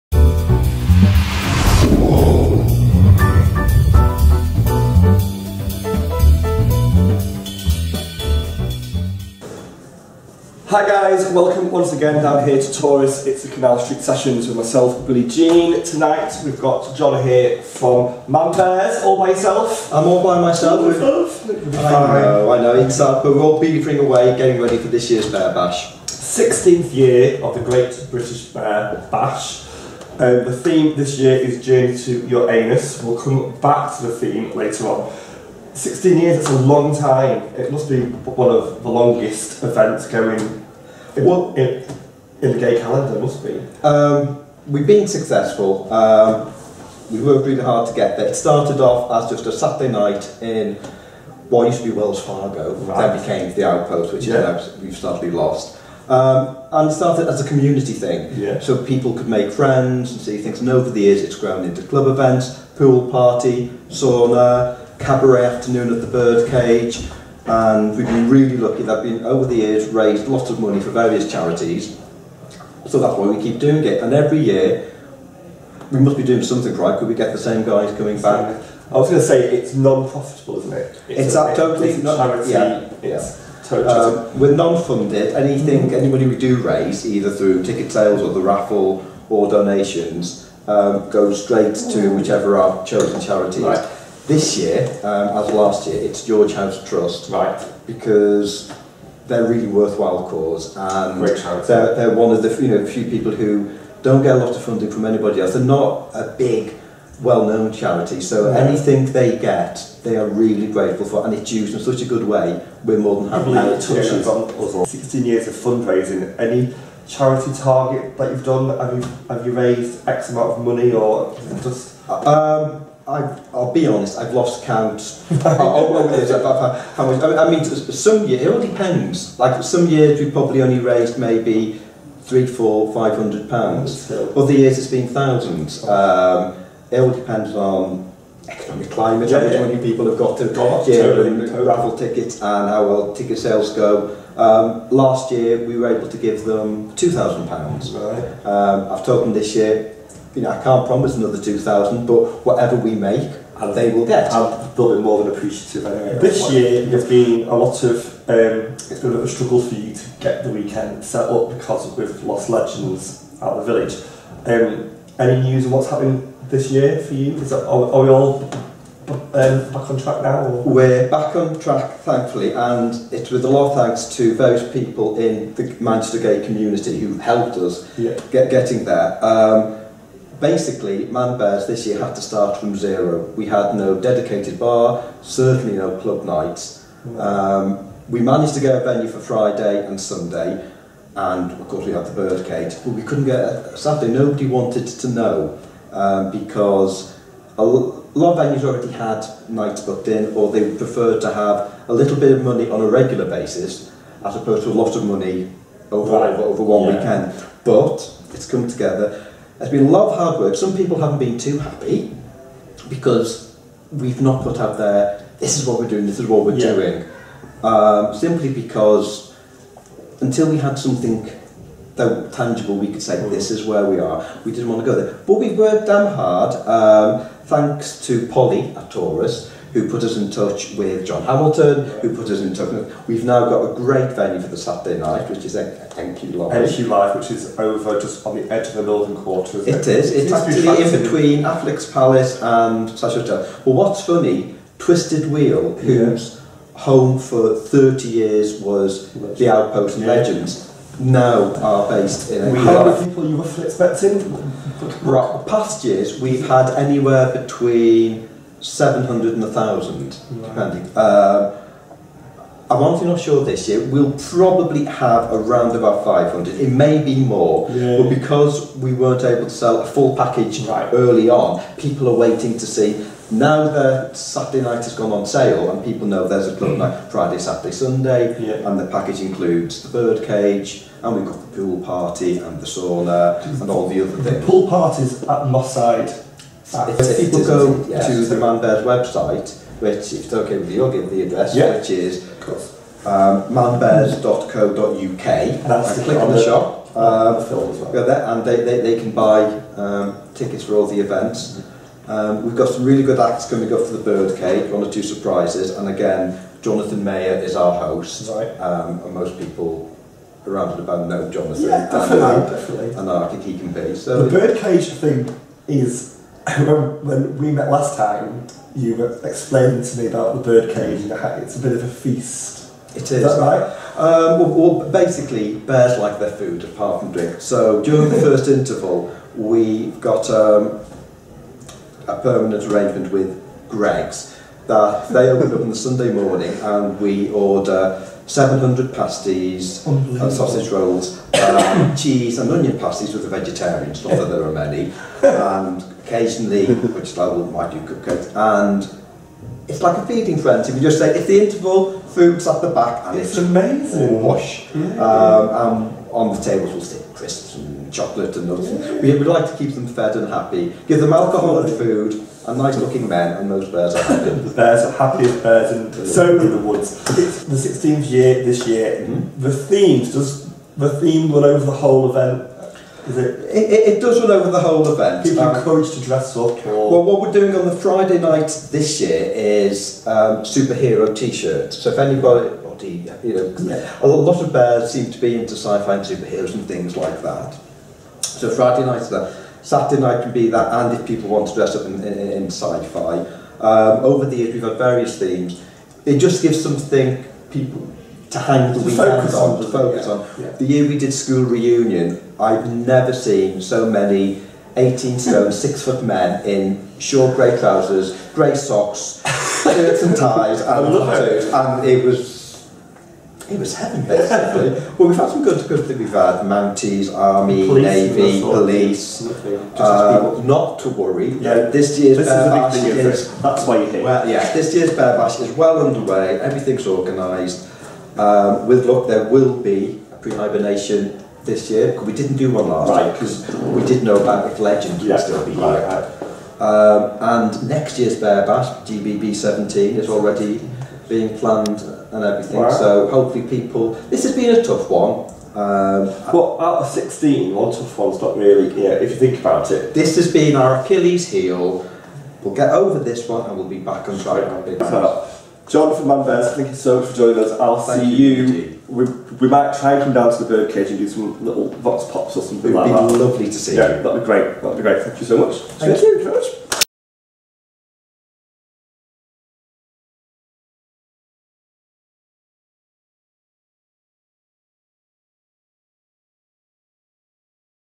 The Canal Street Sessions meets Manbears
Billie Jean chats with John from Manbears about GBBB16